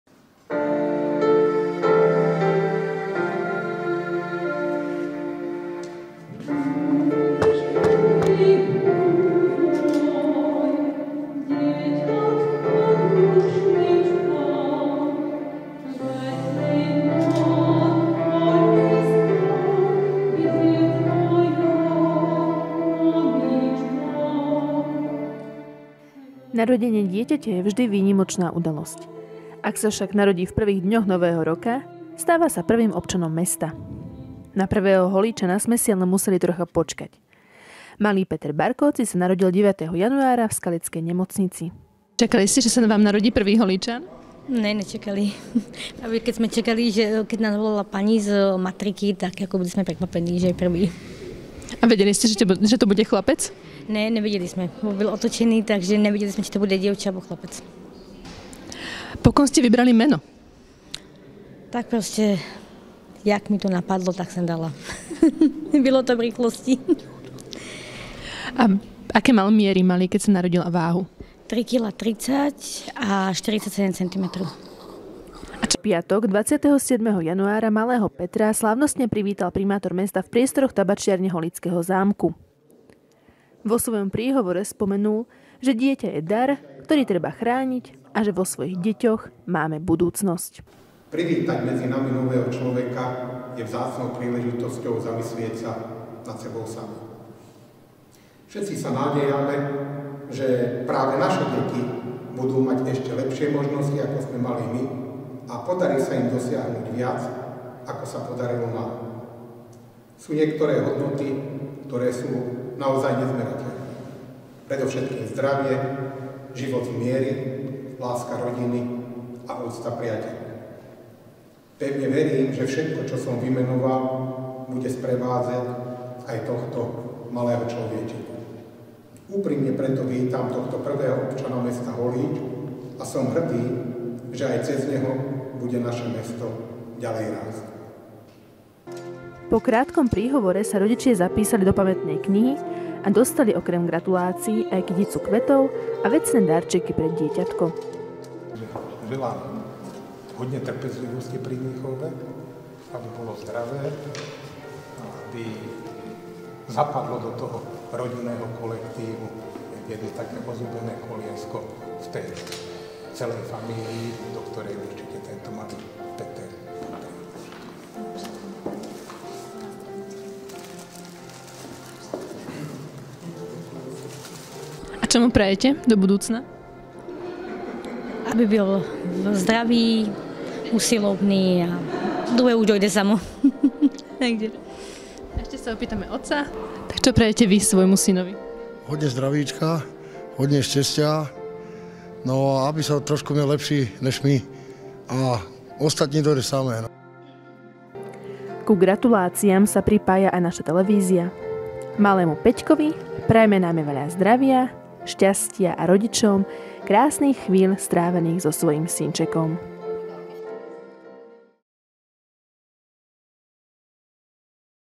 Prvý Holíčan
V Kultúrno-osvetovom centre privítal primátor Zdenko Čambal prvého narodeného Holíčana v tomto roku. Prokopovi a Veronike Barkóczyovým sa 9. januára narodilo už deviate dieťatko.